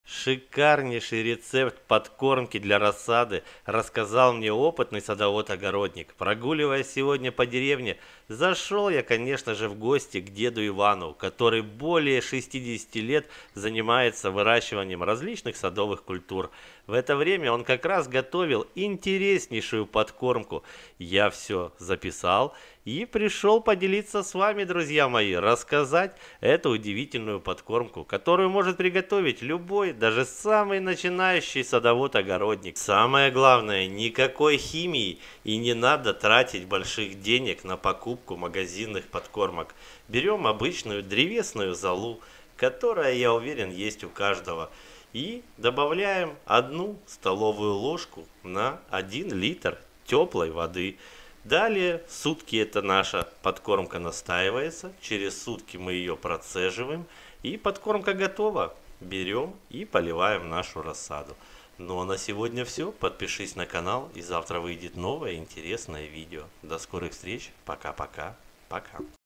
Деда Секрет.1 Ложка и рассада станет жирненькой за одну ночь
Важная подкормка для рассады в марте апреле мае такое удобрение поможет рассаде окрепнуть, также его можно использовать и для всех других растений. Удобрение полностью экологическое, его хватает надолго и в нём действительно очень много полезных веществ! Подкормка рассады!